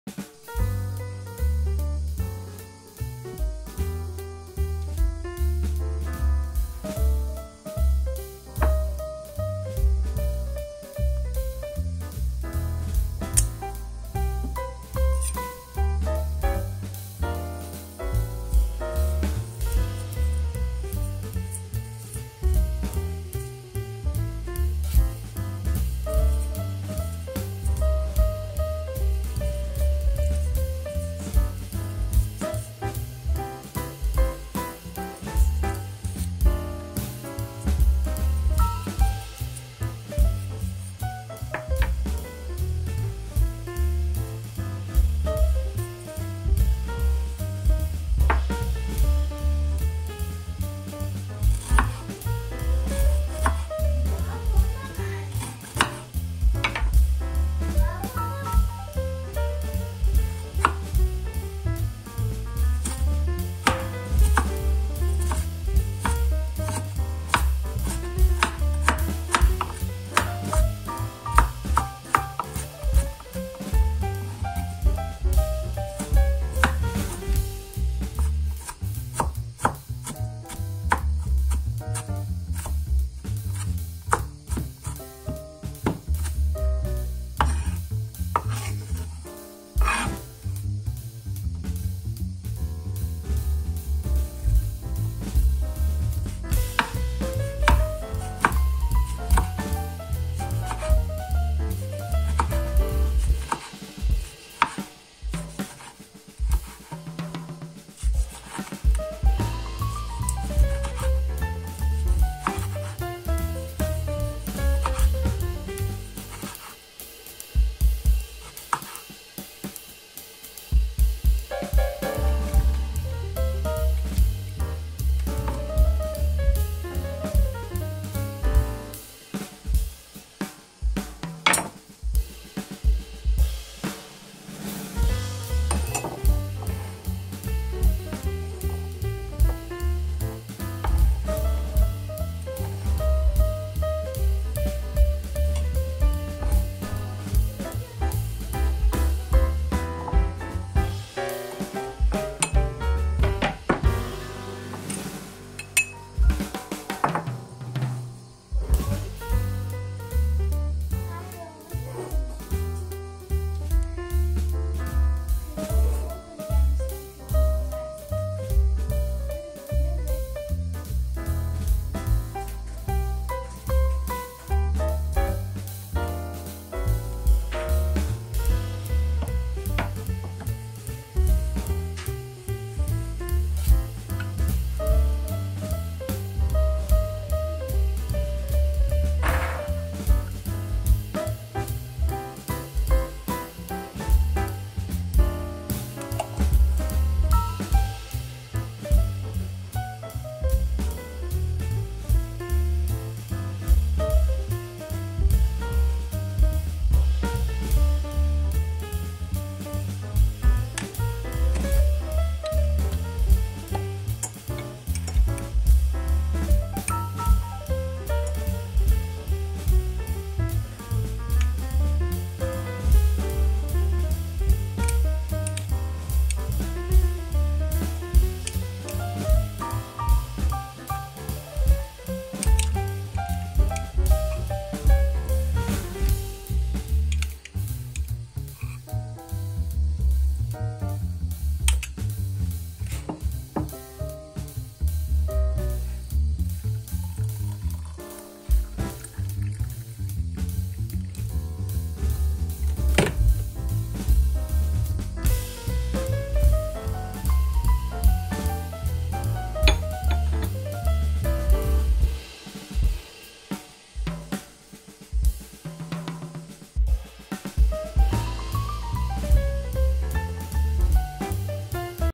Kış İksiri ✅ Virüs Savar👌Zencefil Şurubu Nasıl Yapılır ✅ Bağışıklık Güçlendirici Doğal Antibiyotik
malzemeler
taze zencefil
limon
bal